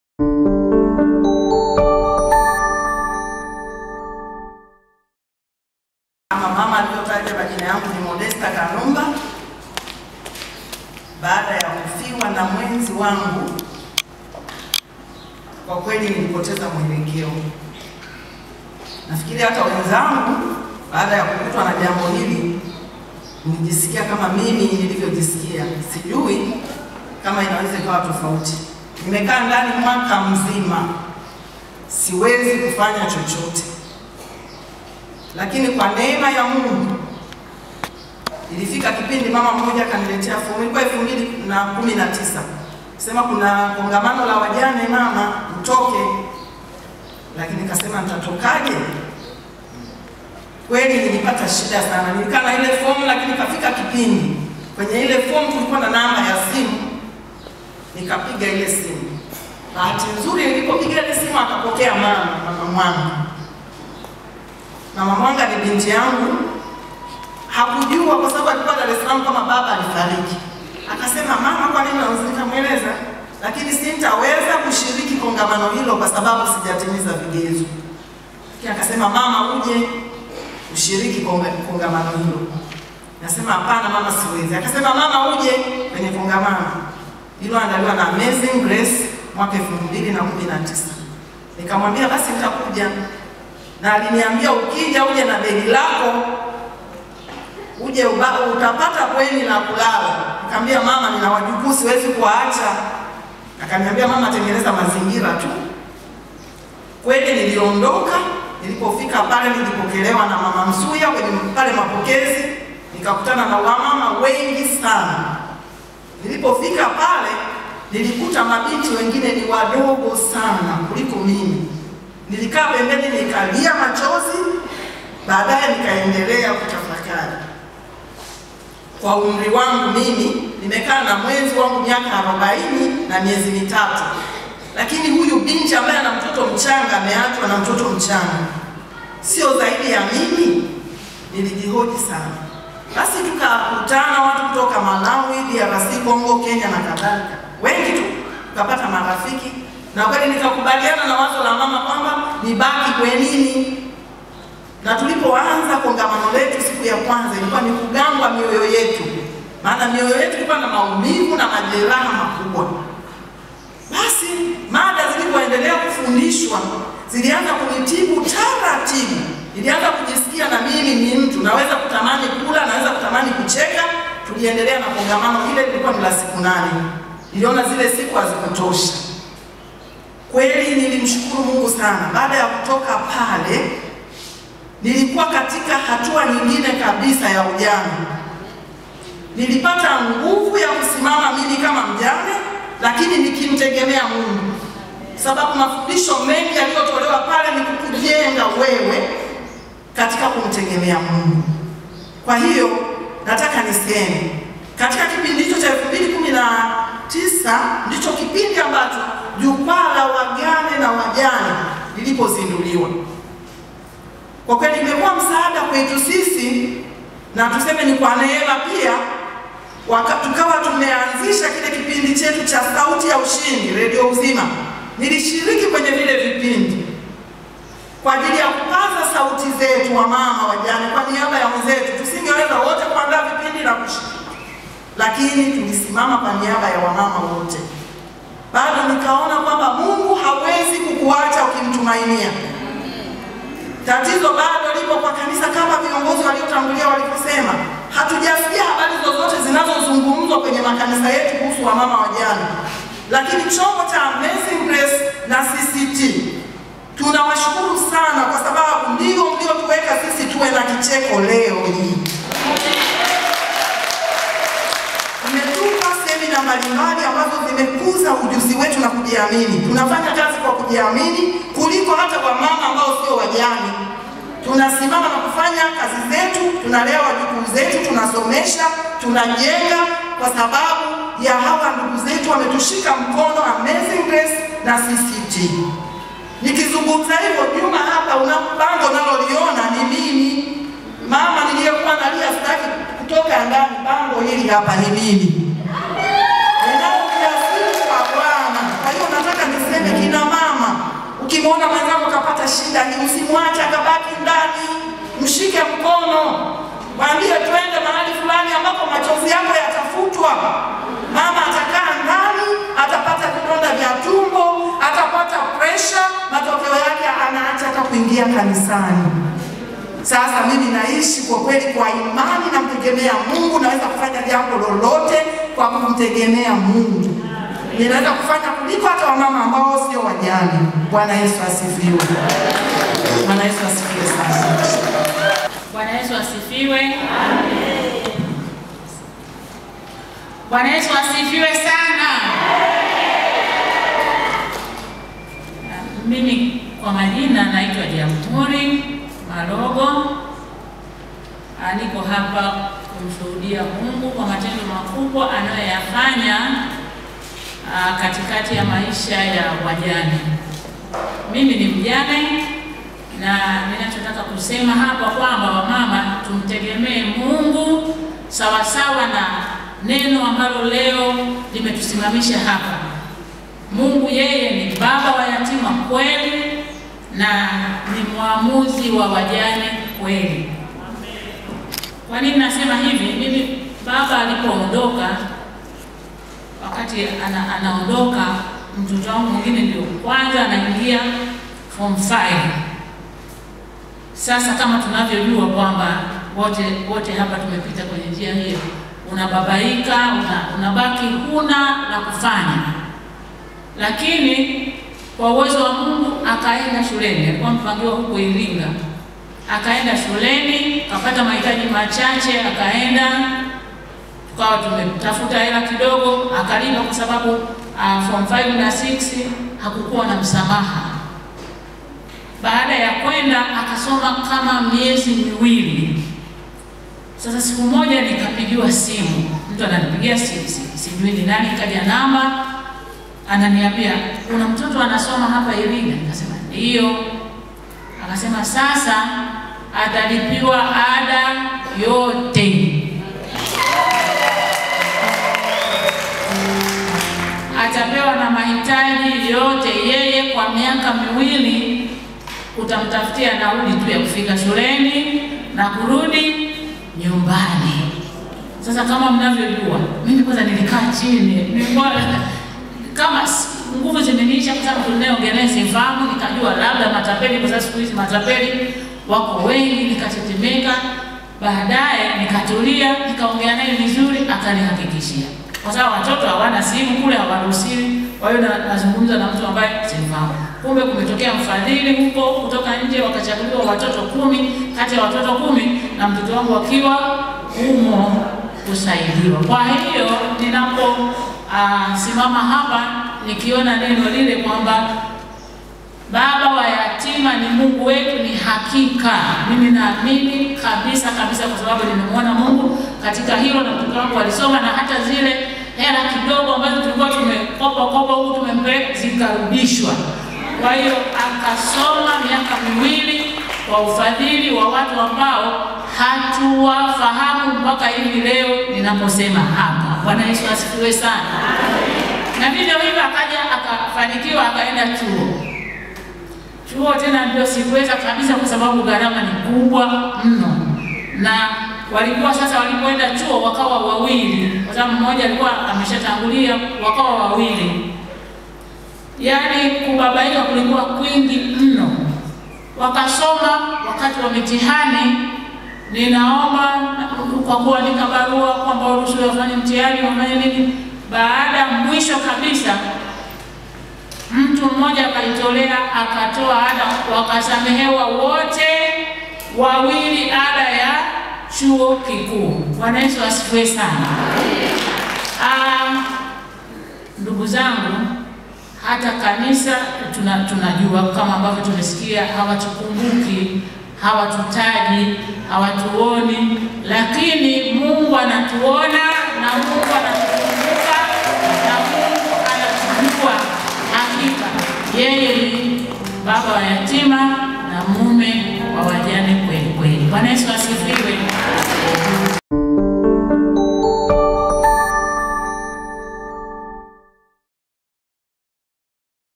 SHUHUDA MAMA MJANE ALIYEKAA NDANI MWAKA MMOJA
Jumuiya ya Kikristo Tanzania (CCT CHURCH HOUSE) Idara ya maendeleo ya Wanawake watoto na jinsia Imeandaa Ibada maalumu kwa kwajili ya maadhimisho ya siku ya Wajane Kikanisa